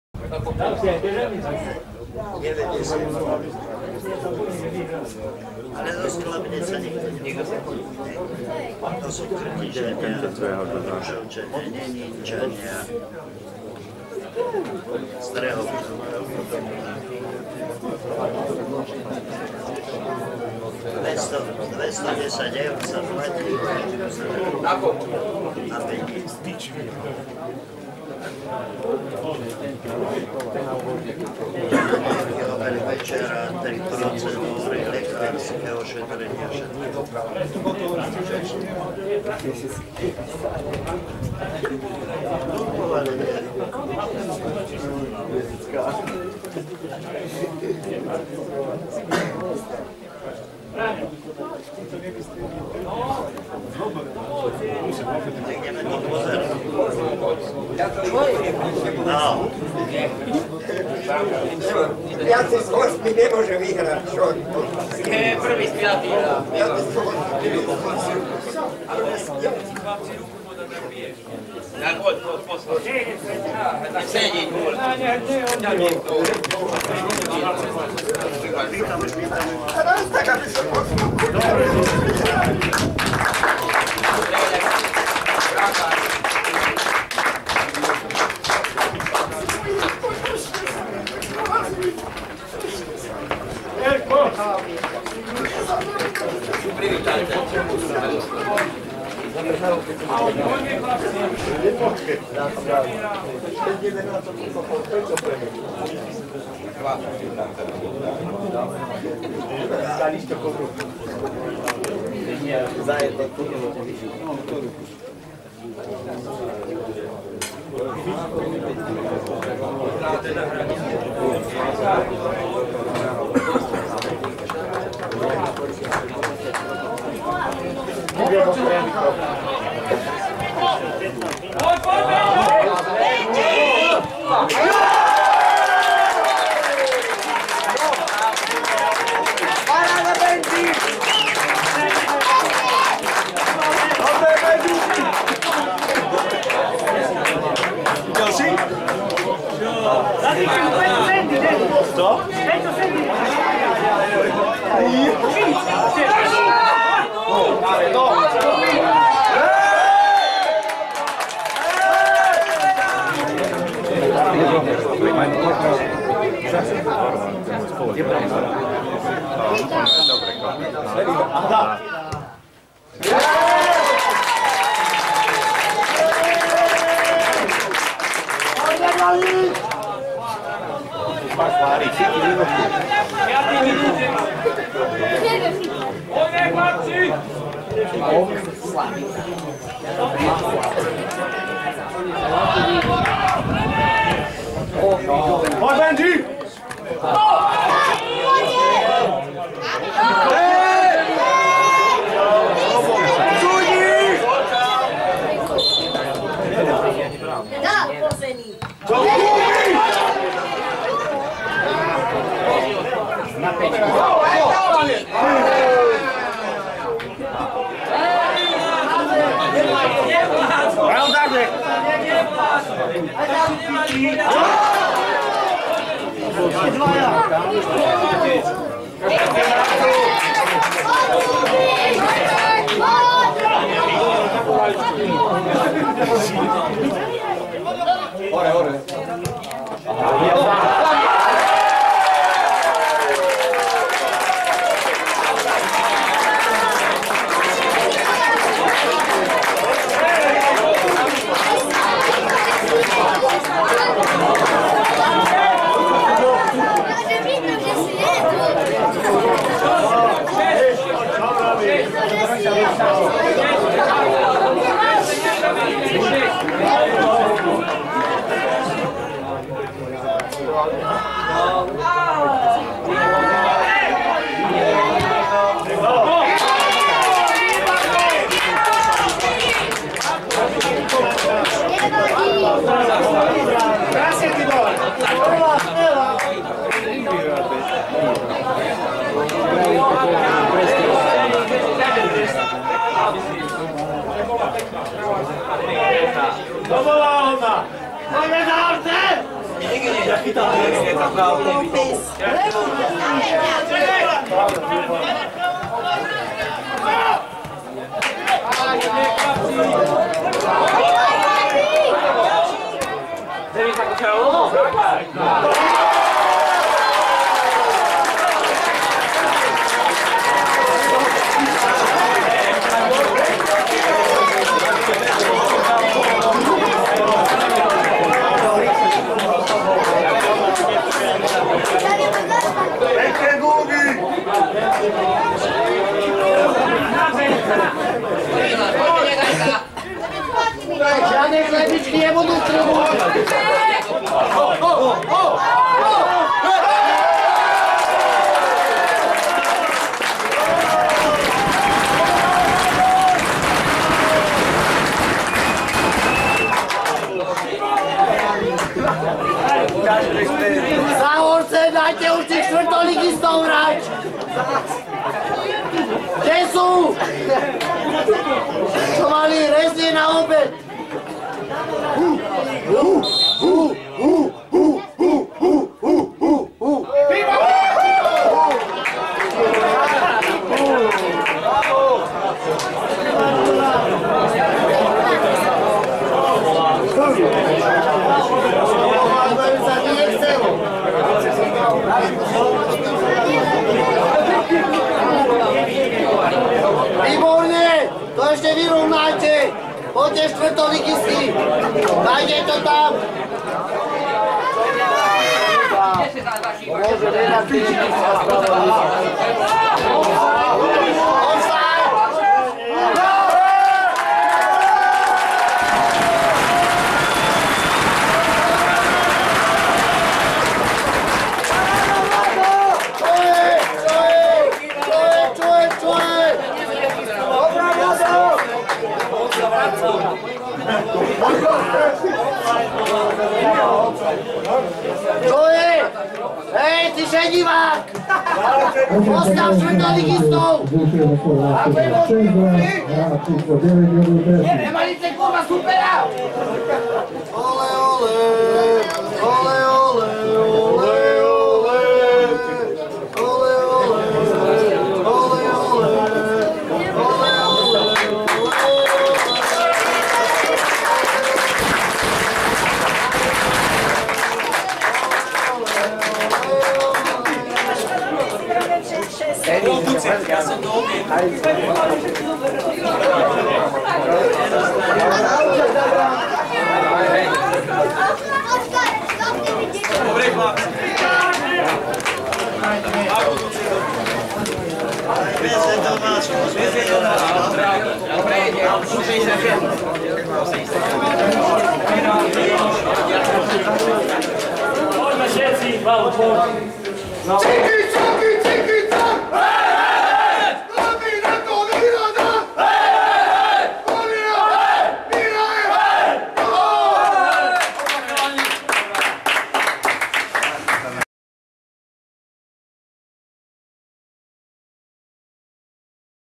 TJ Družstevník Sklabiná - FK Záhorce 6:2
Oblastný futbalový zväz Veľký Krtíš
Futbalový zápas 6. liga, sezóna 2017/2018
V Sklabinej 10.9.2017